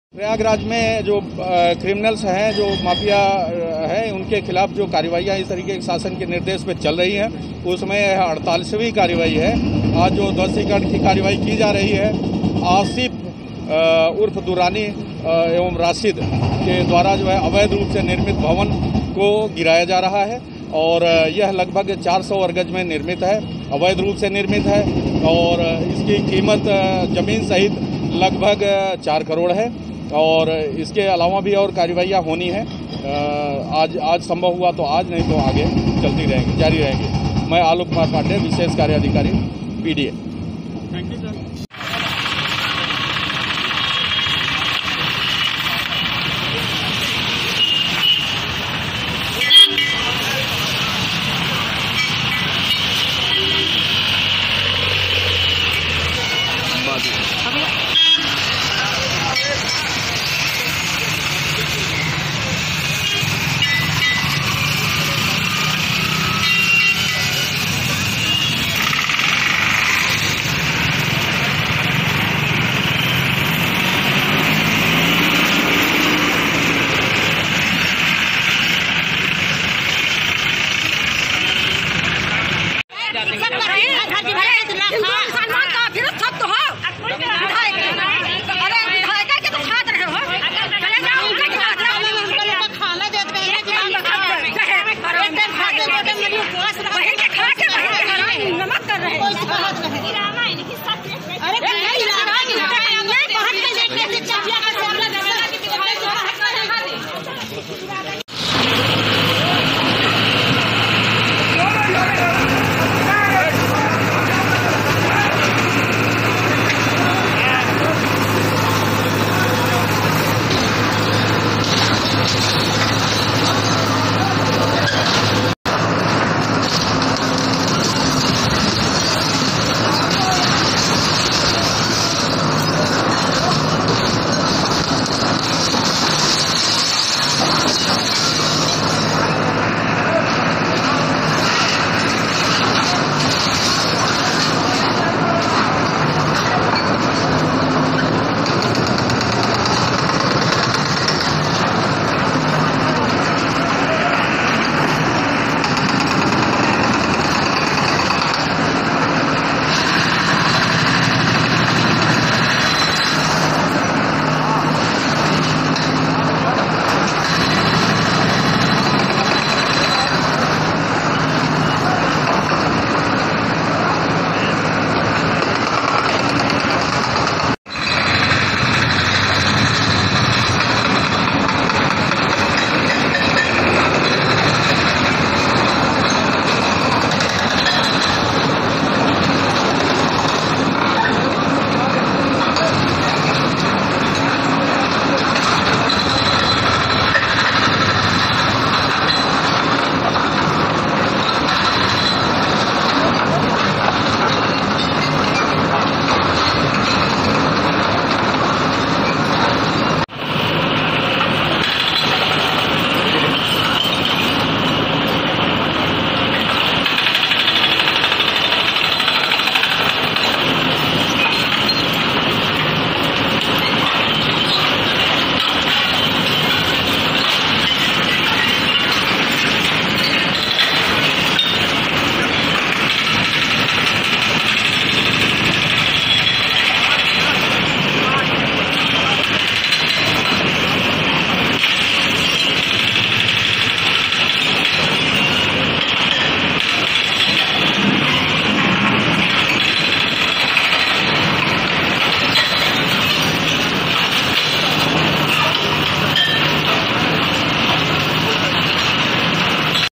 बाहुबली पूर्व सांसद अतीक के गुर्गे के घर पर चला पीडीए का बुलडोजर
उत्तर प्रदेश सरकार का ऑपरेशन माफ़िया जारी है इसी के तहत आज प्रयागराज में 48 वी कार्यवाही विकास प्राधिकरण ने की।विकास प्राधिकण ने माफ़िया अतीक़ अहंमद के करीबी और शूटर आशिफ दुर्रानी के मकान पर बुल्डोजर चला कर ज़मींदोज़ कर दिया।
V/Oअतीक़ के इस गुर्गे ने पीडीए से बिना नक्शा पास कराये आलीशान मकान का निर्माण किया था जिसकी कीमत करोड़ो में है । आज विकास प्राधिकरण ने पुलिस फोर्स के साथ दोनो रास्तों को ब्लाक करके 4 बुल्डोजर लगा कर मकान को गिरा दिया

आशिफ दुर्रानी के ऊपर करेली और धुमन गंज थाने में दर्जनो मुक़दमे दर्ज है। इसके बाद पीडीए की टीम अतीक़ के करीबी आज़म और असाद के अवैध निर्माणों पर बुल्डोजर चलाएगी ये तीनो पुलिस के रिकार्ड में हार्ड कोर क्रिमिनल है और अतीक़ के कारोबार में मदद करते है
बाइट आलोक पांडेय जोनल अधिकारी पीडीए